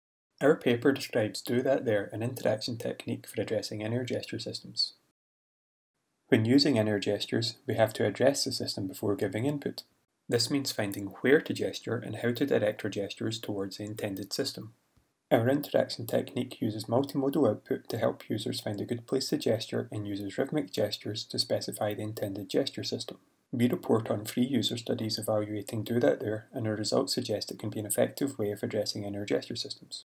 CHI 2016 Preview: Do That, There: An Interaction Technique for Addressing In-Air Gesture Systems
CHI 2016 30-second preview video for our paper.